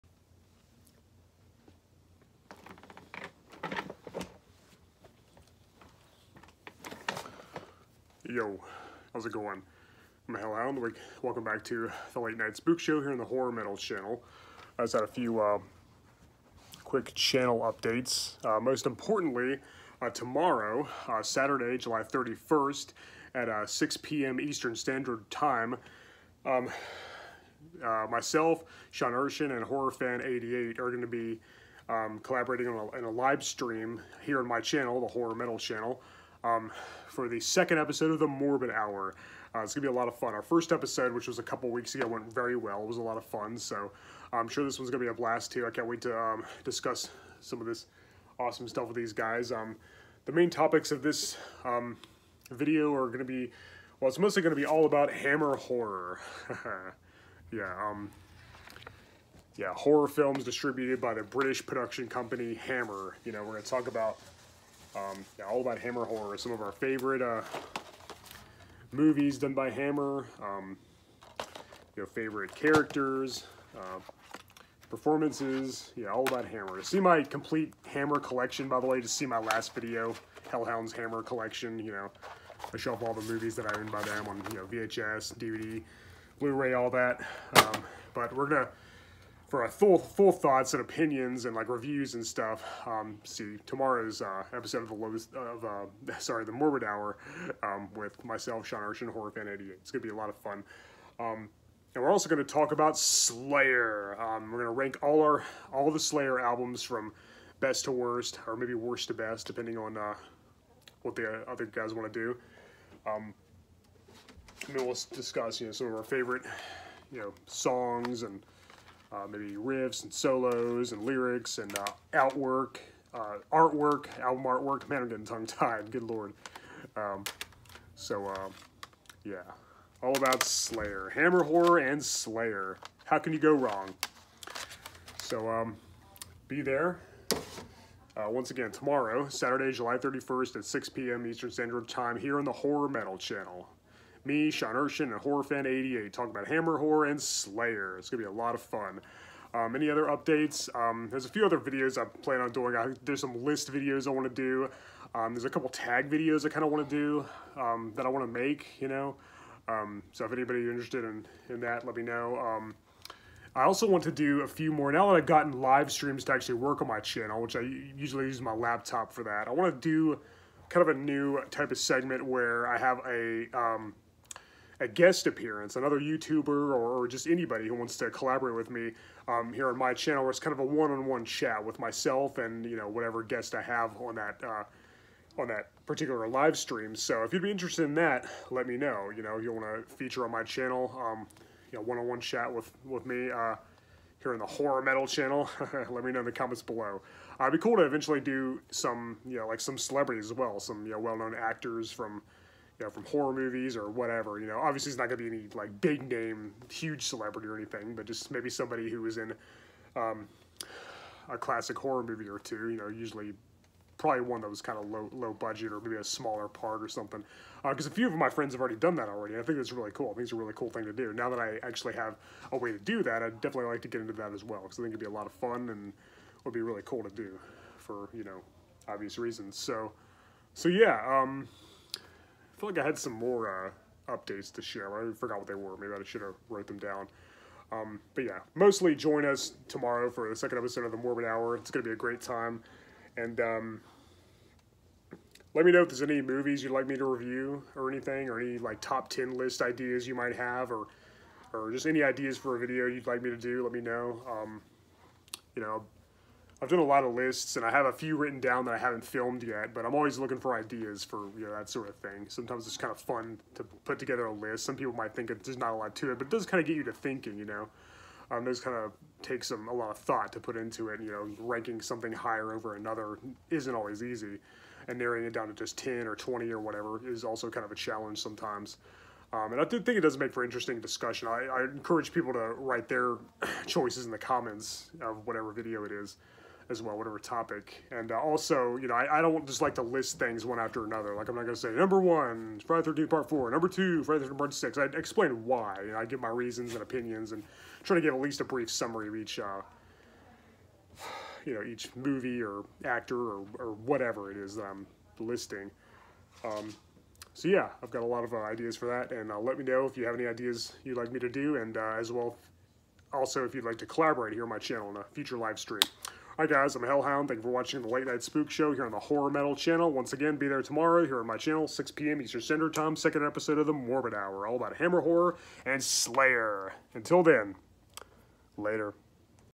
Tomorrow’s Livestream: The Morbid Hour Second Episode (Promo)
Hellhound shares some channel updates, as well as news about the second episode of The Morbid Hour livestream featuring Sean Erschen and Horrorfan88. This collaboration event will premiere on this channel tomorrow at 6pm EST. See you then!